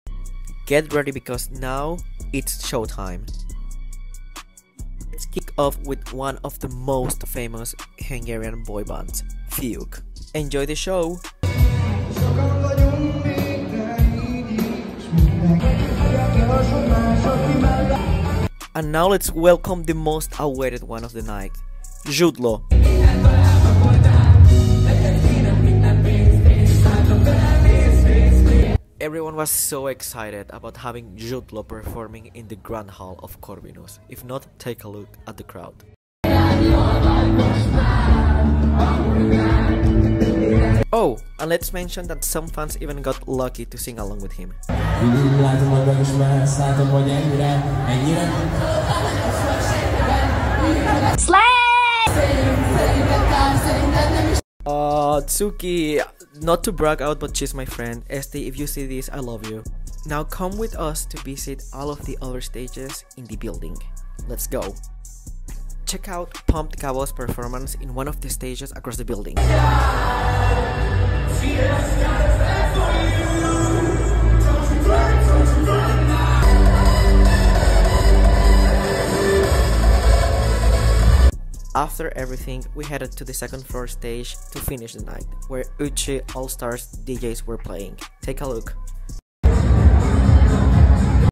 Corvinus Freshmen Vlog - Season 3 Episode 4: Freshmen's Ball, Part 2
This time, Halfid will take us on a journey through one of the biggest parties of the semester -- Freshmen Ball organized by ÖCSI.